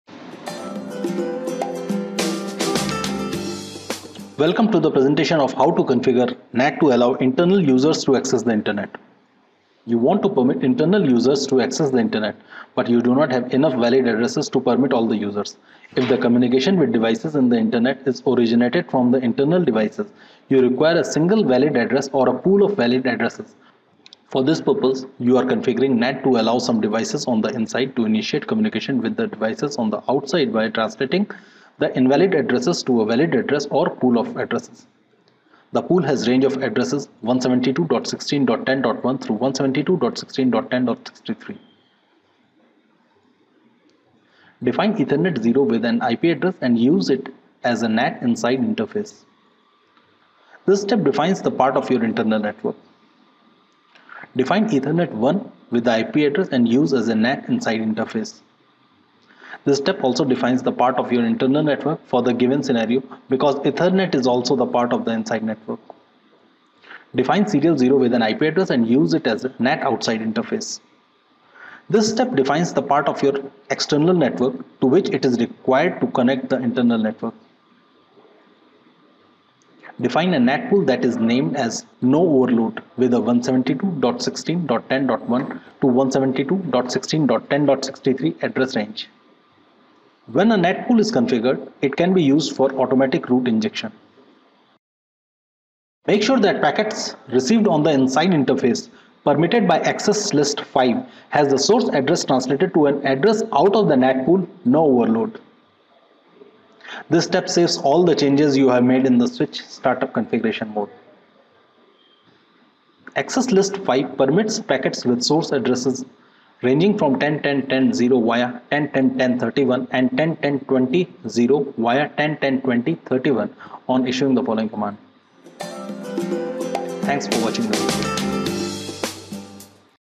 CCNA-How to configure NAT to allow internal users to access the Internet.mp4
This video shows how to configure NAT to allow internal users to access the Internet